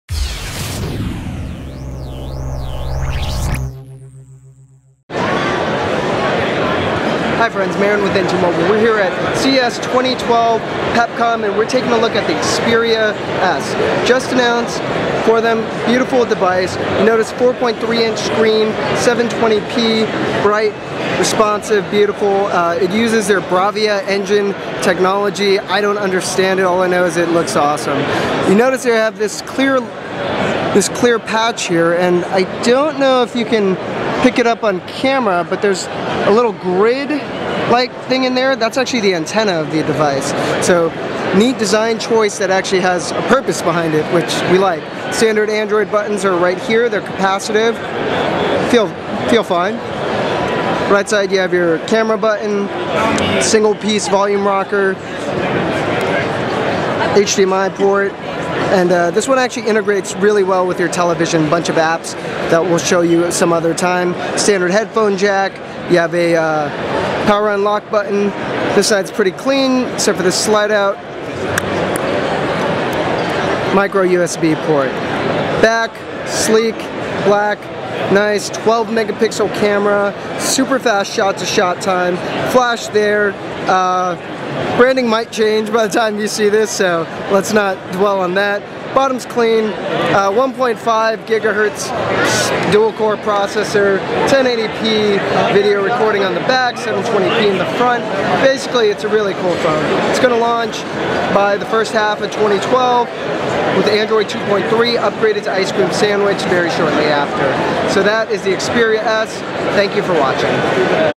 Sony Xperia S hands-on from CES 2012
From CES 2012, we get a look at the Sony Xperia S and see how this 720p HD screen Android phone stacks up against the competition.